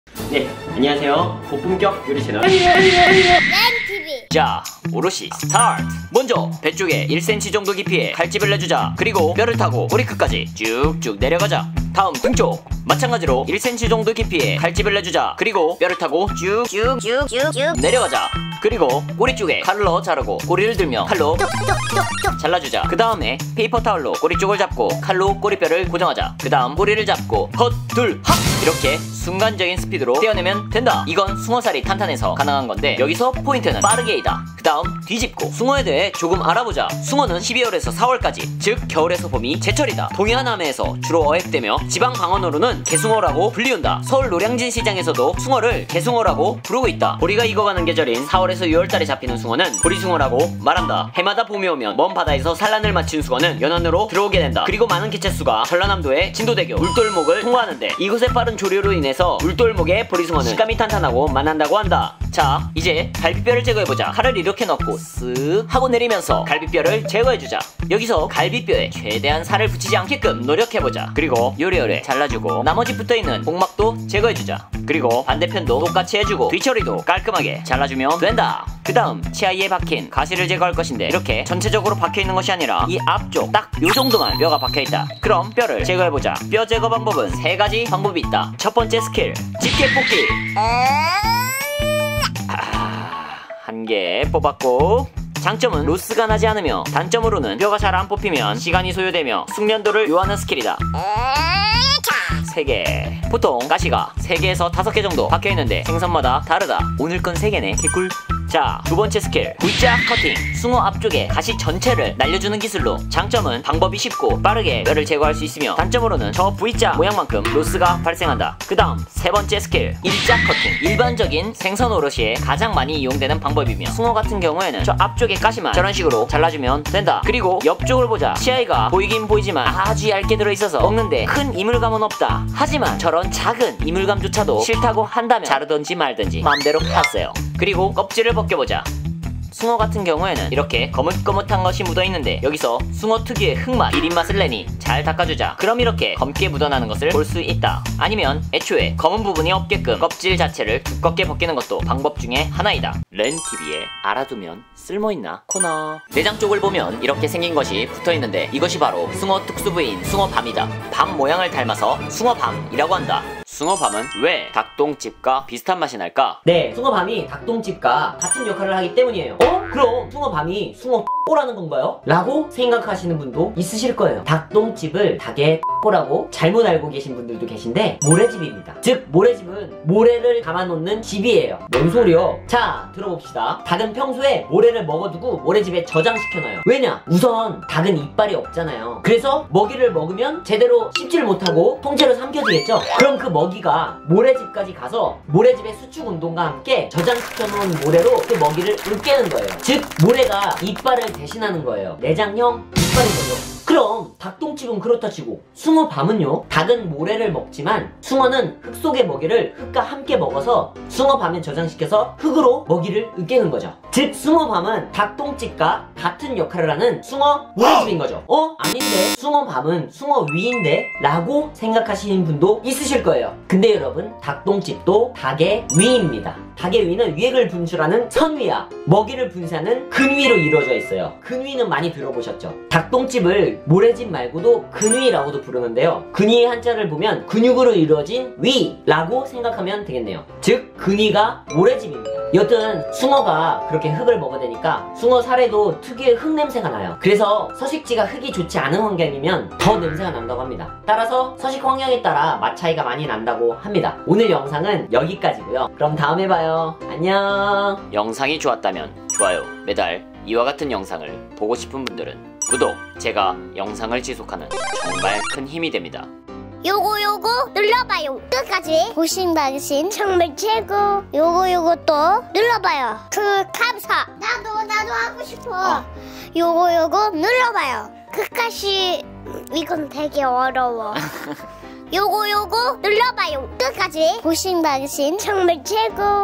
숭어도 똥집이 있다고? 맛있는 특수부위와 닭똥집과의 관계! flathead mullet 숭어오로시
숭어 오로시를 알려드립니다~^^ go go go~
숭어 특수부위인 숭어밤~
숭어밤은 왜 닭똥집과 비슷한 맛이 나는지?? 그 이유를 알려드립니다~!!